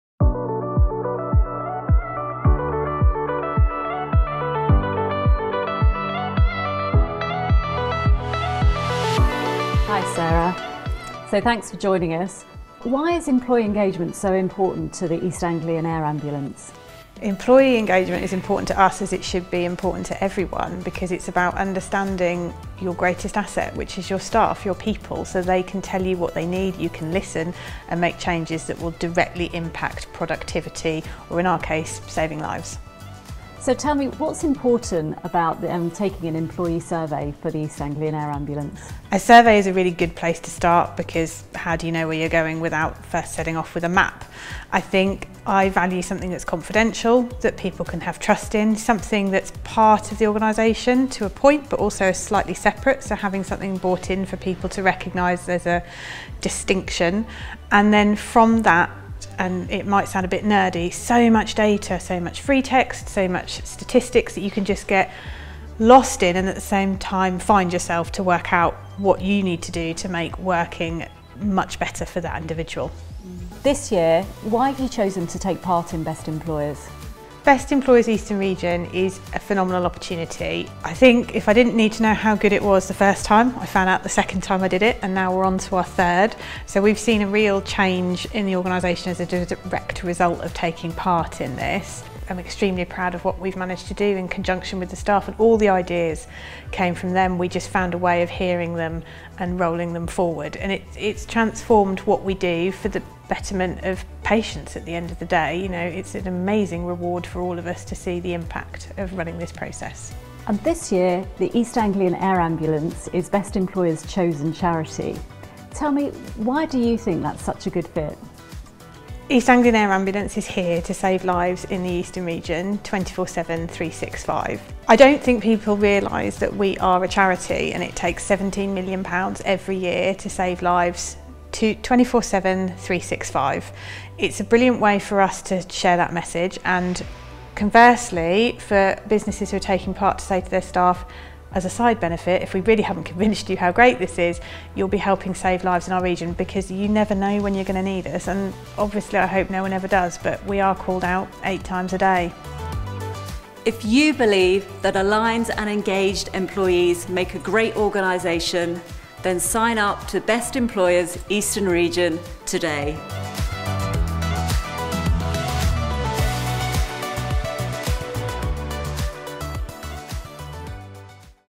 Why does East Anglian Air Ambulance value the Best Employer programme?
Through Best Employers 2023, we're donating any funds we raise to the East Anglian Air Ambulance, a charity that exists to save lives by delivering highly skilled doctors and critical care paramedics by air or car to seriously ill or injured people in the region. They are a life-saving charity that is only kept airborne thanks to incredible supporters.